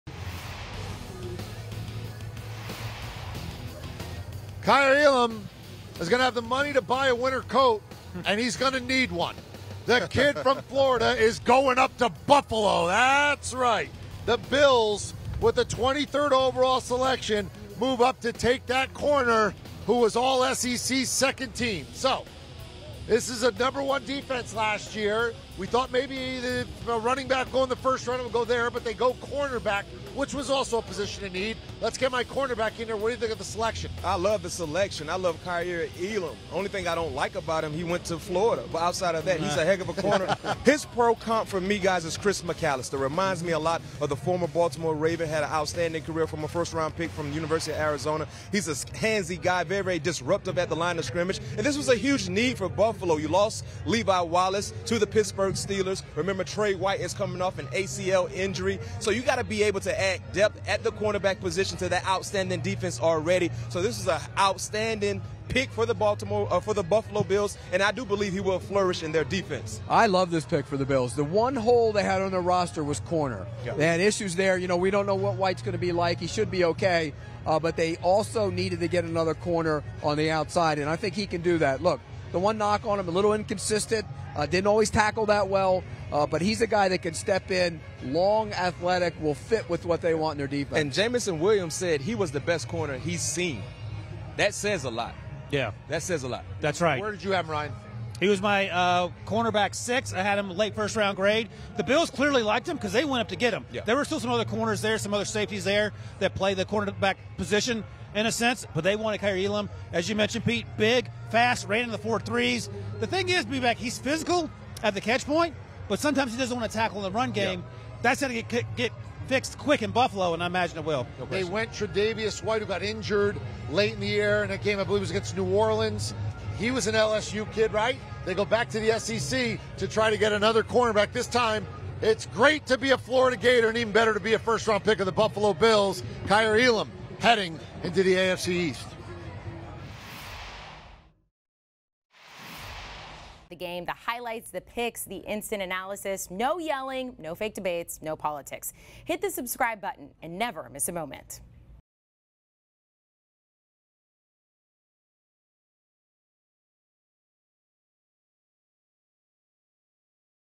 Bills fill MAJOR NEED with CB Kaiir Elam at No. 23 | 2022 NFL Draft Grades
Pete Prisco joins CBS Sports HQ to grade the Bills selecting Kaiir Elam with the No. 23 pick in the 2022 NFL Draft.